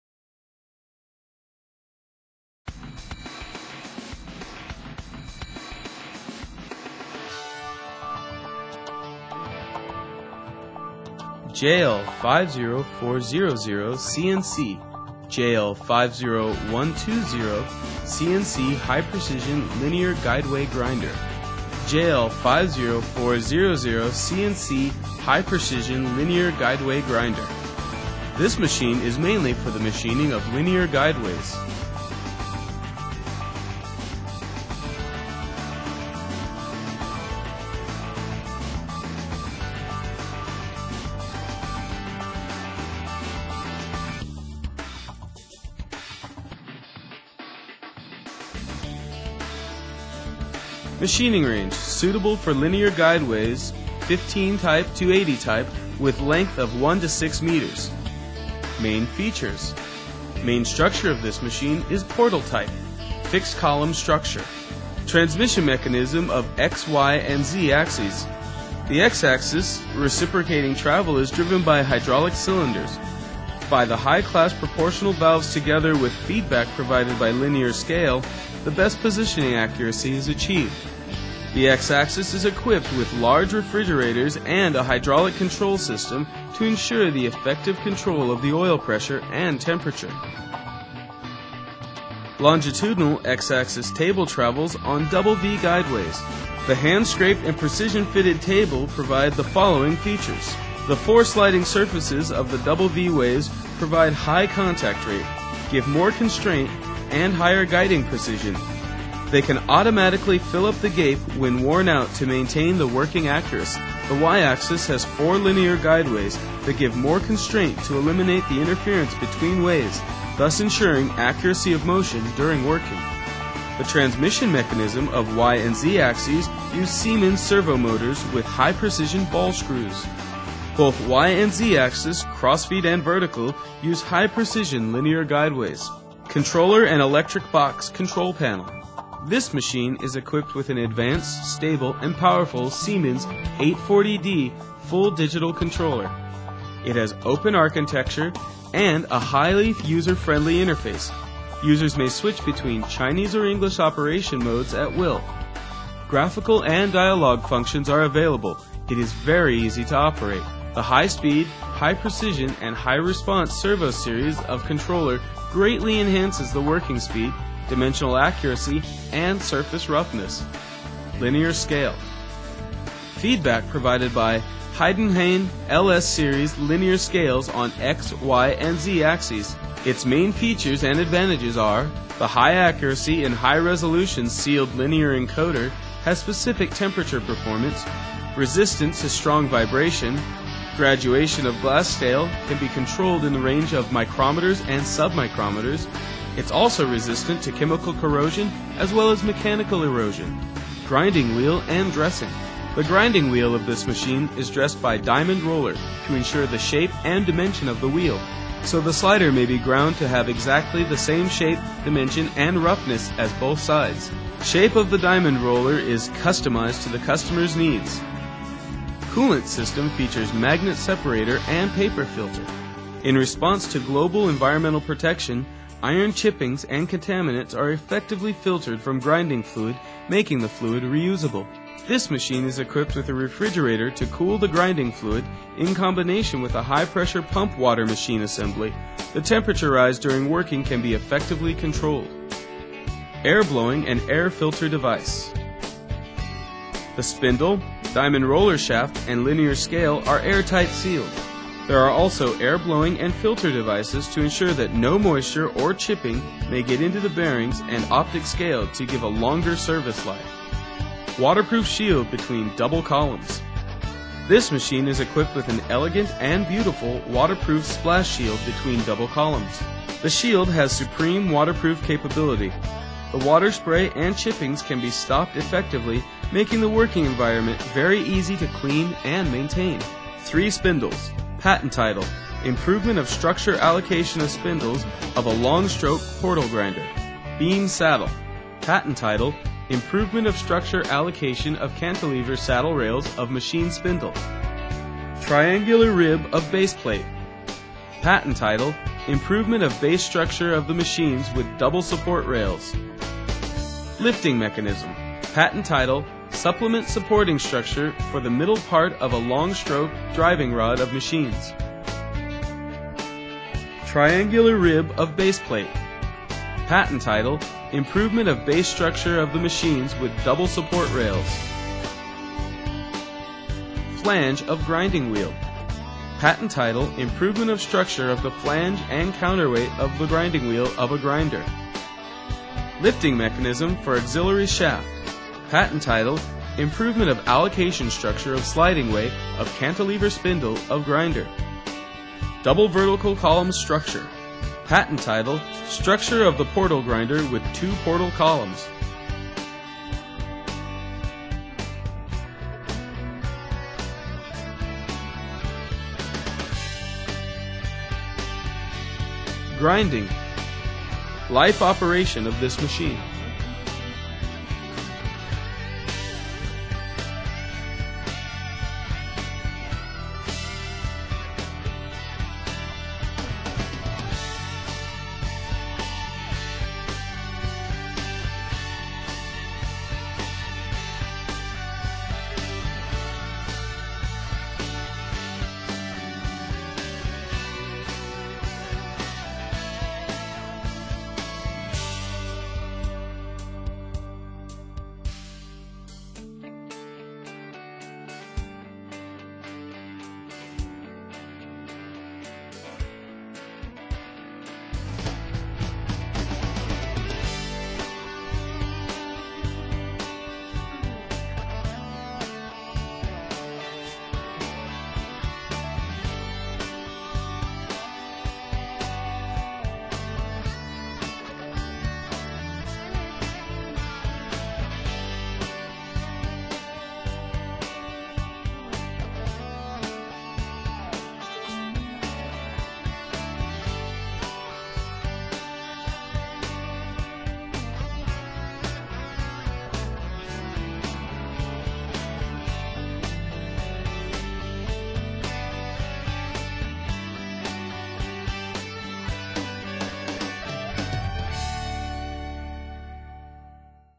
Taiwan Joen Lih, CNC Double Column Type Surface Grinder,CNC Linear way Surface Grinder.
JL-50400CNC / JL-50120CNC High Precision Linear Guide Way Grinder
This machine is mainly for the machining of linear guide ways.
Main Features:
Main structure of this machine is portal type fixed column and beam twin vertical columns structure.
Advantages:
The portal type structure is thermally symmetry, which has the merits of extremely high structure rigidity and low thermal effect so the extremely stable accuracy may be maintained for a long time.
    The solid and thick base may enhance the rigidity of this machine.
The portal type design greatly shortens the unsupported part of the spindle head and gives higher rigidity.
The main structure is made by FC30 high quality cast iron, its tempered (sometimes also called annealed) and natural aged to thoroughly release the internal stress for the purpose of working accuracy.
Range: Suitable for linear guide ways 15 type ~ 80 type with length of 1~6 meters.